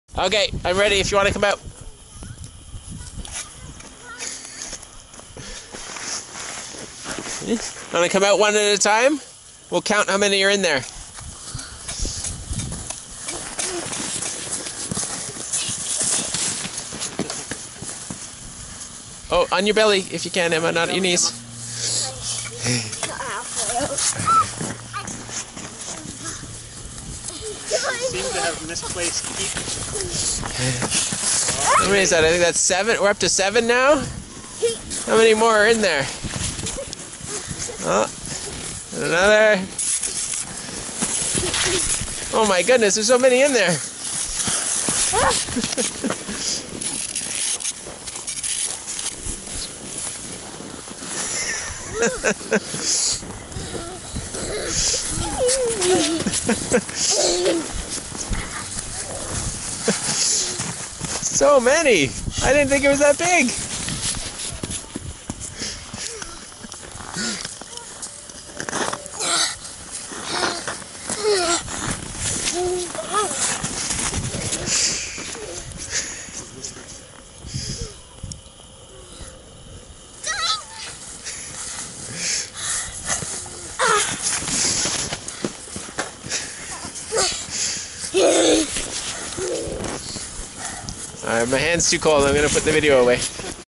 How many kids are in that snow fort?
Is it a snow fort, or a clown car?  How many kids can fit inside of that awesome quinzhee?